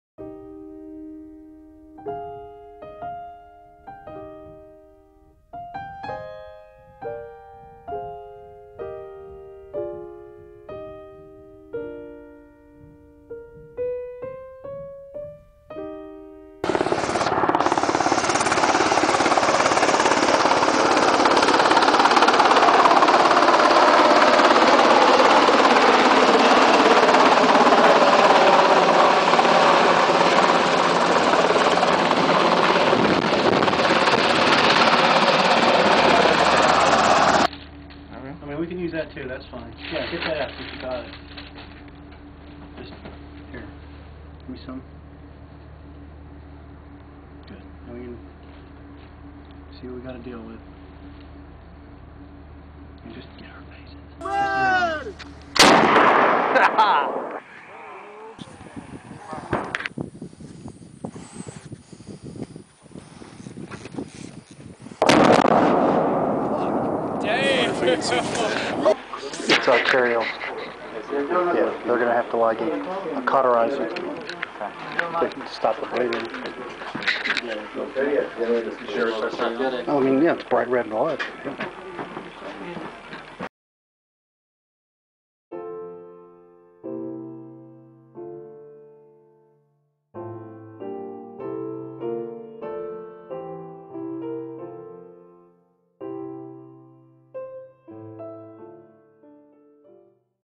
My Journey as a Combat Medic 2nd book trailer
Second book trailer for the book My Journey as a Combat Medic: From Desert Storm to Operation Enduring Freedom